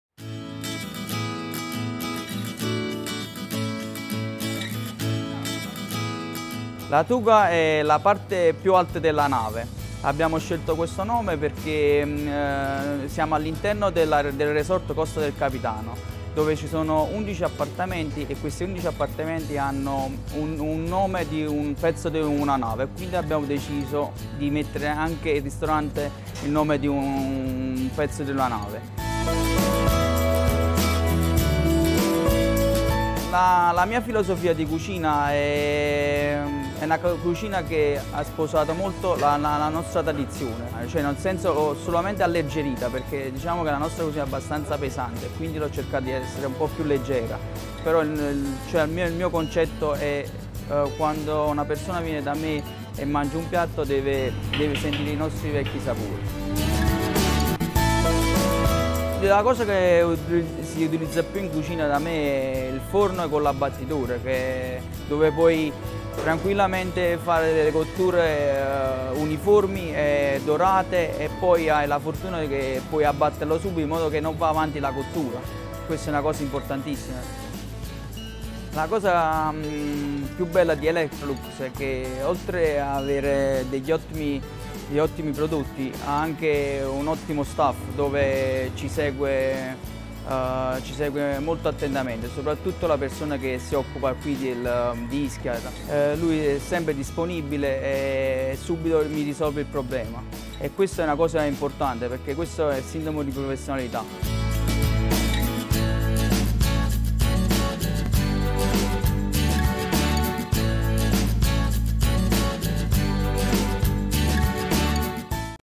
La Tuga Restaurant - Ischia (Naples), Italy | Electrolux Professional
Inside the prestigious "Costa del Capitano" Resort, owned by the entrepreneur Francesco Polito, stands the La Tuga Restaurant, led by Chef Gianluca D'Ambra.
Guests are offered an enchanting journey by land into the Ischian tradition, which begins in the Mediterranean garden-park surrounding the restaurant. This ends on the table with the refined creations by Chef D’ambra, also thanks to the support of the modern technology by Electrolux Professional.